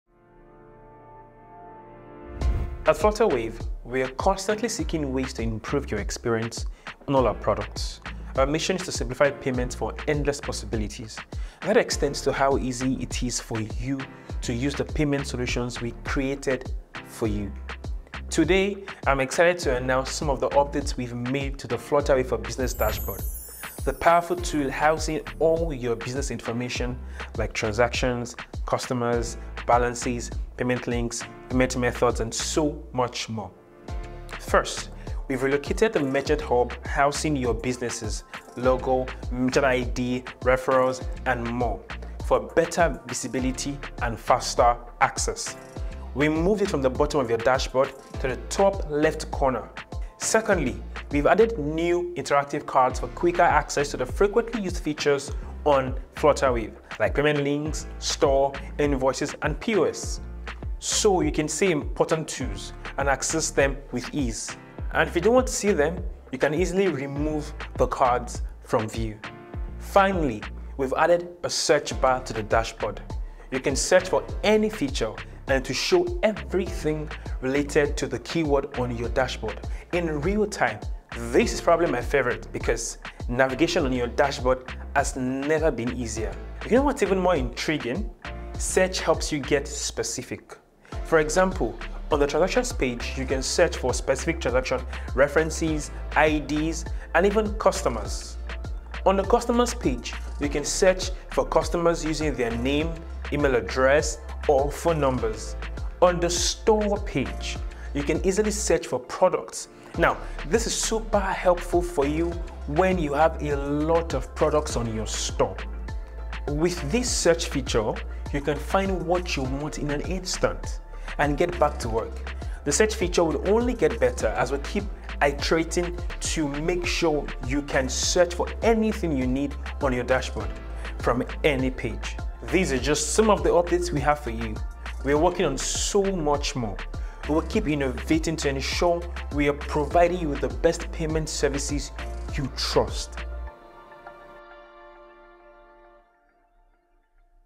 See What's New On The Flutterwave Dashboard
We've made some exciting changes to the Flutterwave dashboard for faster access and easier navigation. We relocated the merchant hub and added interactive cards for frequently used features and a powerful search bar.